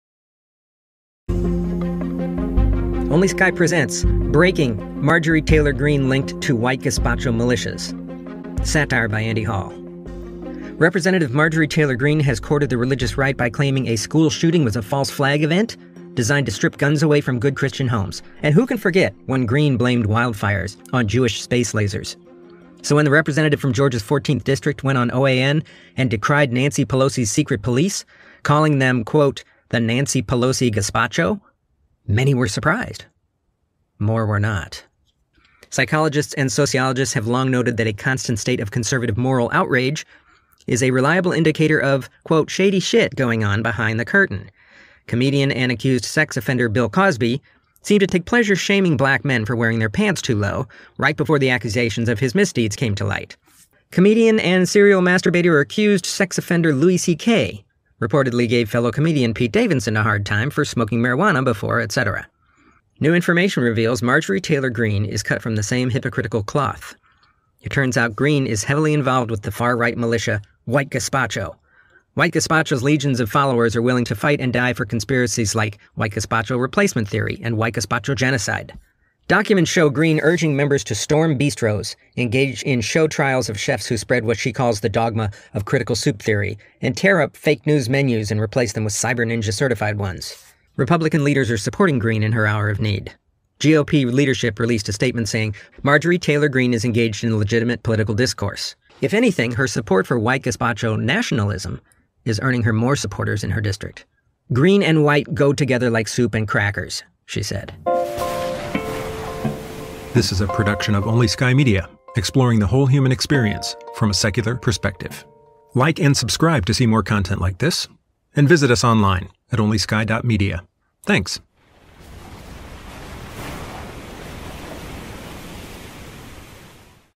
Marjorie Taylor Greene linked to White Gazpacho militias | Satire by Andy Hall
Representative Marjorie Taylor Greene claims Nancy Pelosi's secret police, the Gazpacho, are a threat.